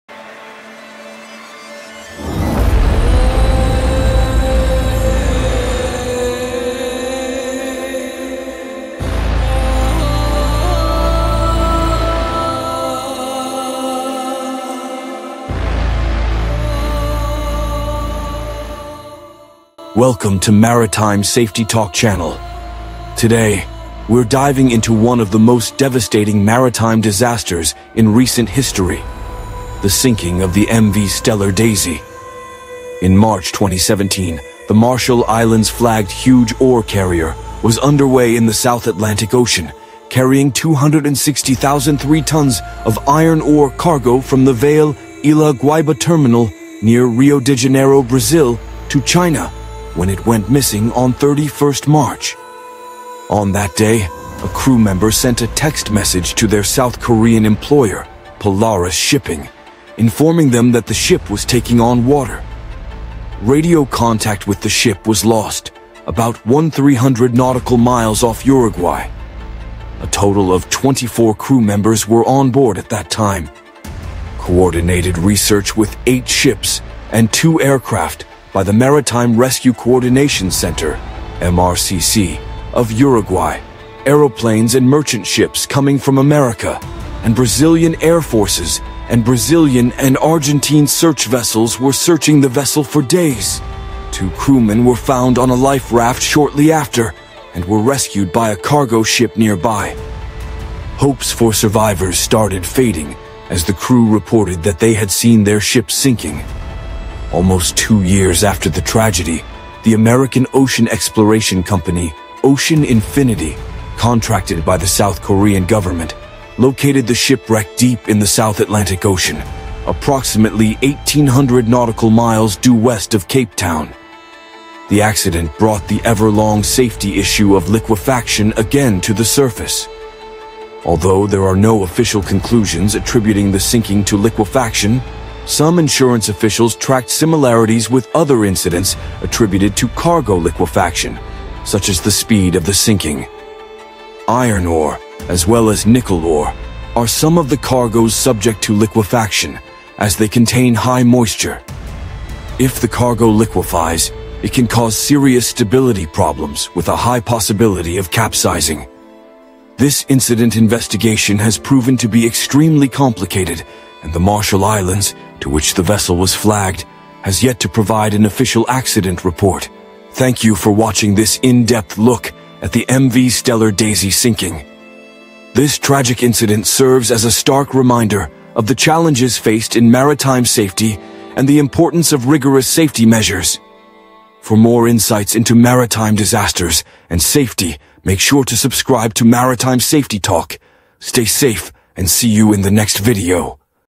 "The Harrowing Sinking of MV Stellar Daisy: What Happened?"
Join us on a journey to unravel the harrowing mystery behind the tragic sinking of MV Stellar Daisy in this gripping video. Explore the events leading to the maritime disaster, understand the challenges faced by the crew, and delve into the aftermath of the incident. Discover the untold story behind the loss of MV Stellar Daisy and the lessons learned from this tragic event.